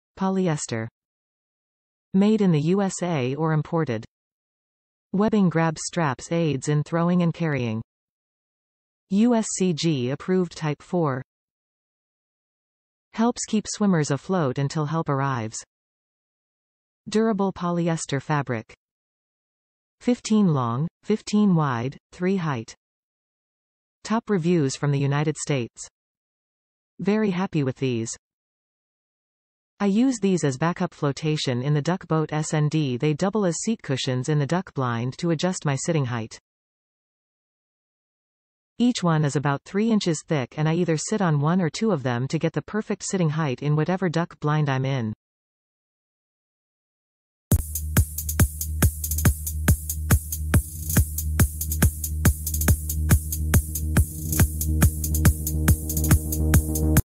Airhead Type IV Throwable Cushion (Multiple Colors Available)
Polyester
Made in the USA or Imported
Webbing grab straps aids in throwing and carrying
USCG approved Type IV
Helps keep swimmers afloat until help arrives
Durable polyester fabric
15" long, 15" wide, 3" height

Very happy with these.
I use these as back-up flotation in the duck boat snd they double as seat cushions in the duck blind to adjust my sitting height. Each one is about 3” thick and I either sit on one or two of them to get the perfect sitting height in whatever duck blind I’m in.